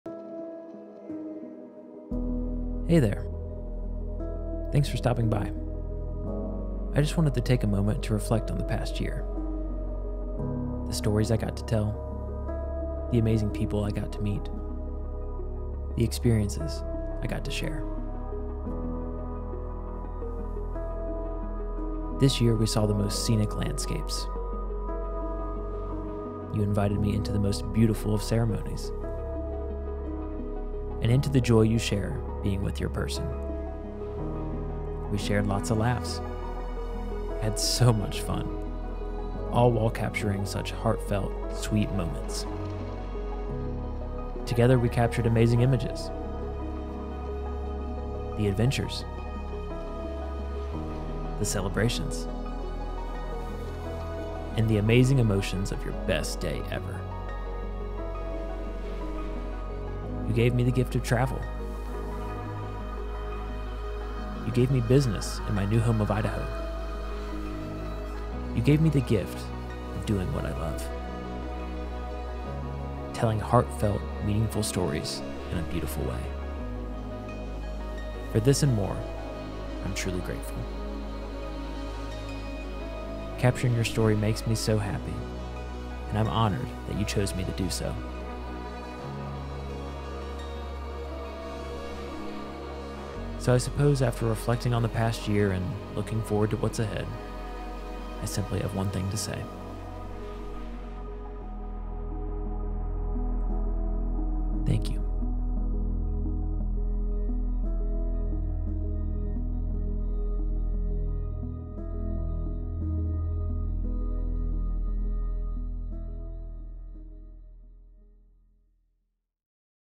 Odyssey's 2022 Retrospective Video
2022 was an amazing year for Odyssey Film and Photo. This year brought about my move to Idaho. It welcomed adventure films to my offerings. And I've been able to travel all over the country doing what I love. Similar to last year, I wanted to create Odyssey's 2022 retrospective video to highlight this awesome, adventurous year.